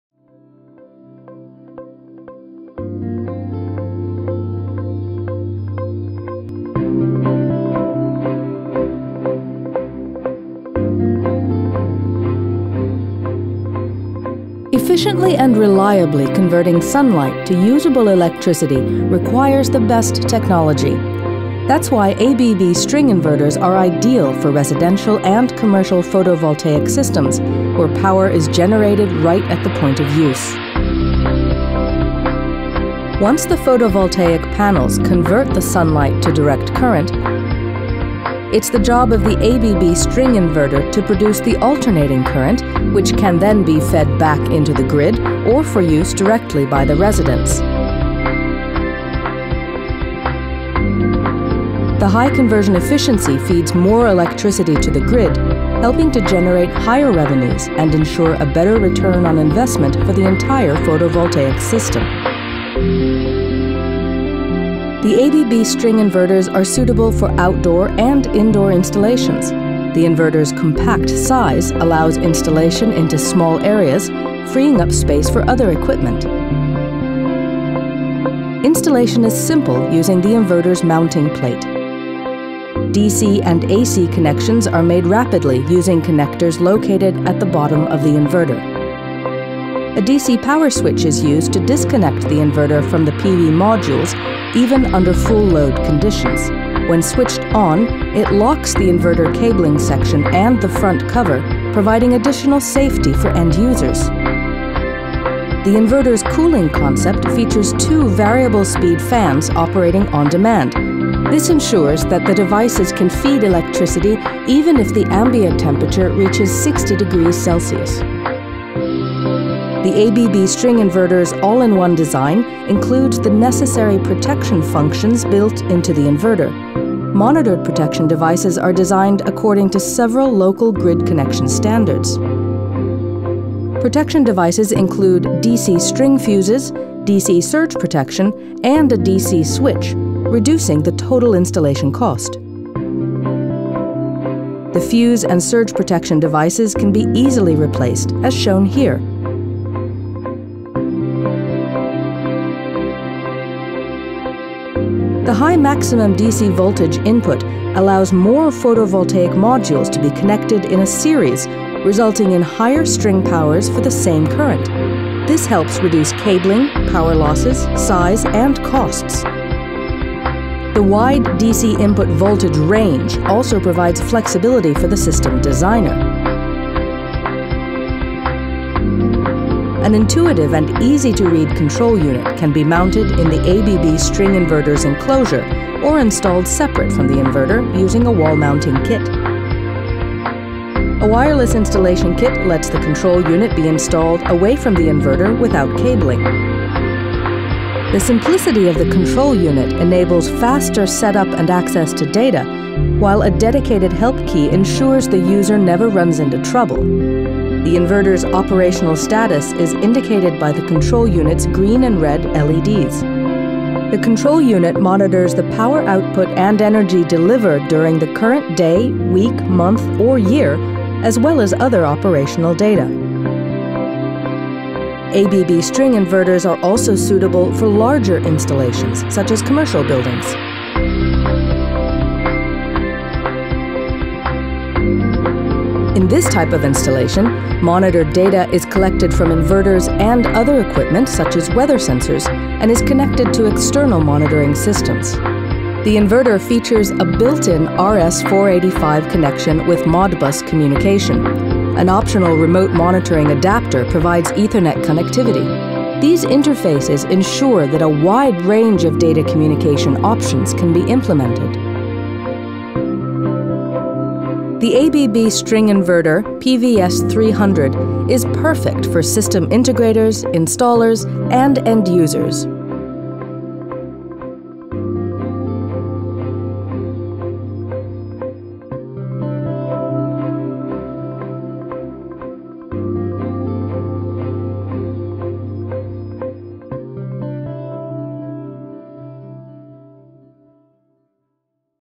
ABB String Inverter
ABB's compact string inverter offers high total efficiency in all conditions meaning that more electricity can be fed to the public grid, resulting in higher revenues for the end-user and a faster payback on investment.
Ideal for residential, commercial and industrial buildings, the ABB string inverter is suitable for both outdoor and indoor use and is available from 3.3 to 8.0 kW.